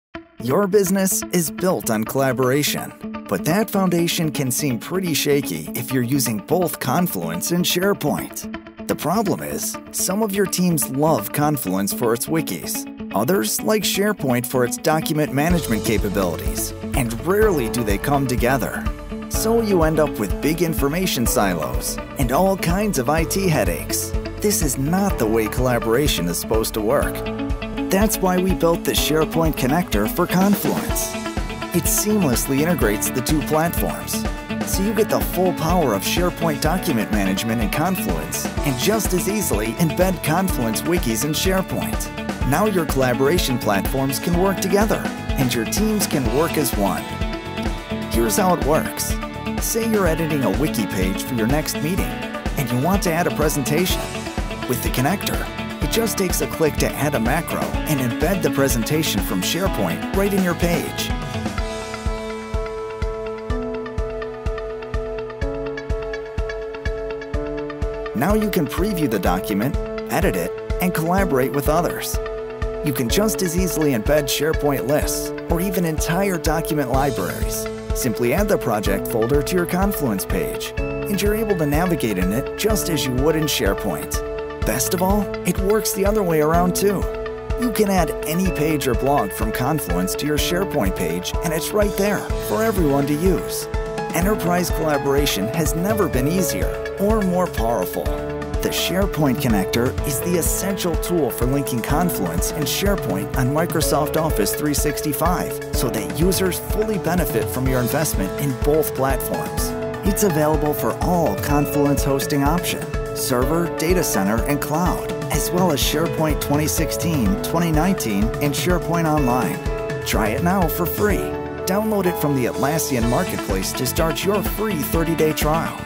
SharePoint Connector for Confluence
Integrate Confluence with SharePoint, and discover a powerful new way of collaborating.